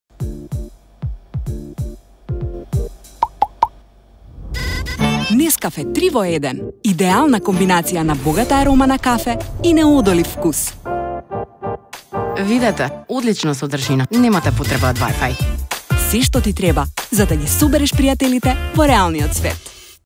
NESCAFÉ 3in1 - за повеќе моменти во живо
Зошто ви е потребен интернет кога можете да го поминете денот во друштво на вашите сакани пријатели? Уживајте во заедничките моменти со NESCAFÉ 3in1 - вистинската комбинација на богата арома на кафе и неодолив вкус. Секогаш.